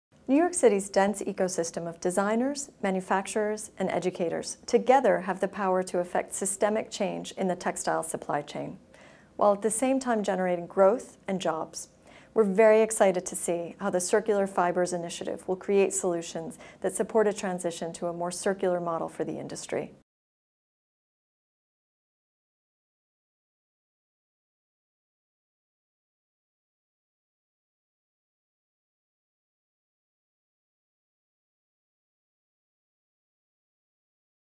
Kate Daly, SVP of NYC Economic Development Corporation, on a Circular Economy for Fashion
Kate Daly, SVP, NYC Economic Development Corporation on the potential power of the wide range of stakeholders brought together by the Circular Fibres Initiative to transform the textiles sector through the principles of the circular economy.